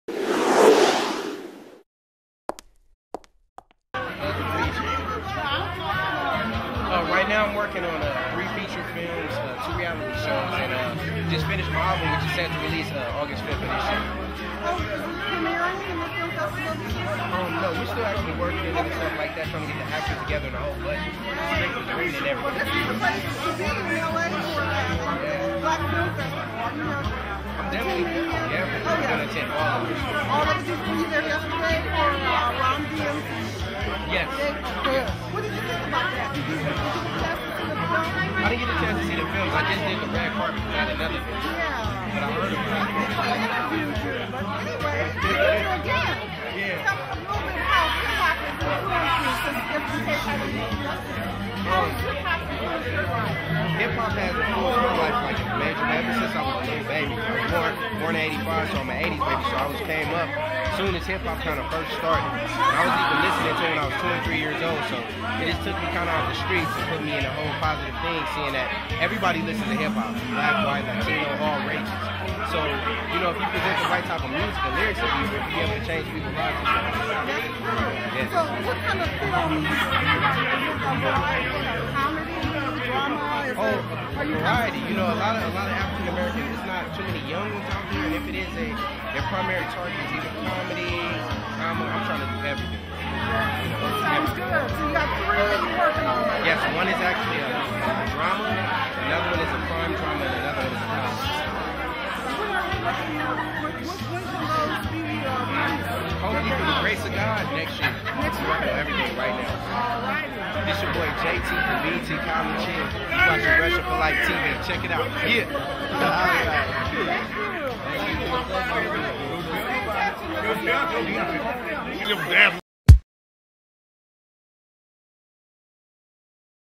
College Hill Star JayTee
Tracy Tluv talks to BET's JayTee, about his career and up coming projects